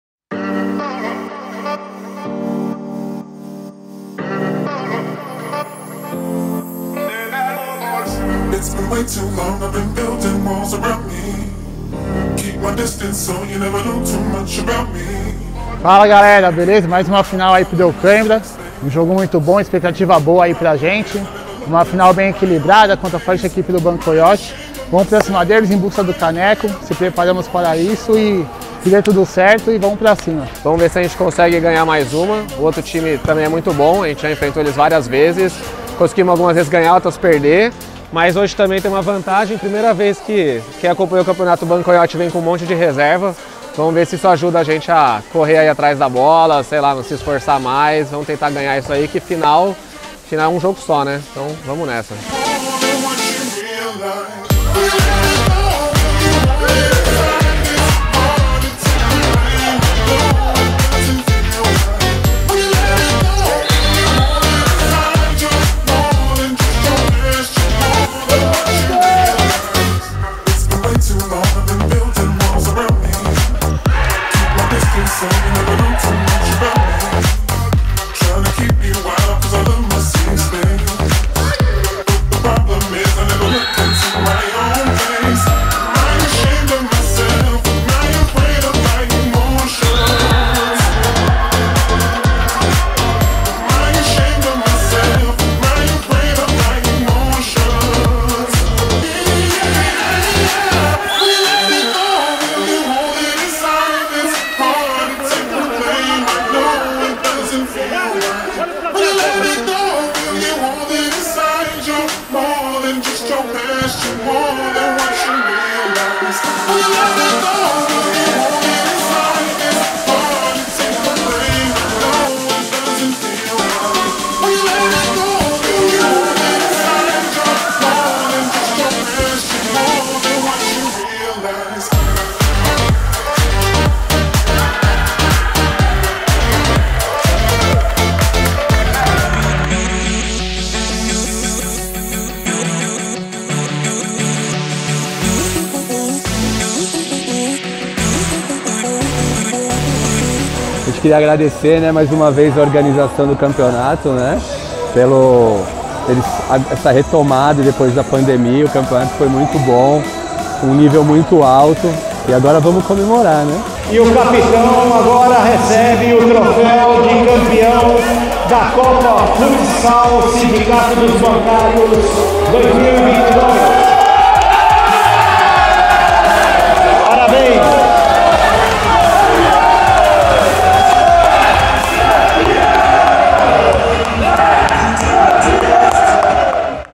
Final da Copa Futsal dos Bancários 2022
A partida final aconteceu no último sábado, 17 de setembro, no Clube da APCEF SP em Interlagos.

O Sindicato dos Bancários agradece a todas as equipes inscritas e parabeniza a Deu Kaimbra/ Família, equipe vencedora.

O placar foi de Deu Kaimbra / Família 07 x 01 Bancoiote.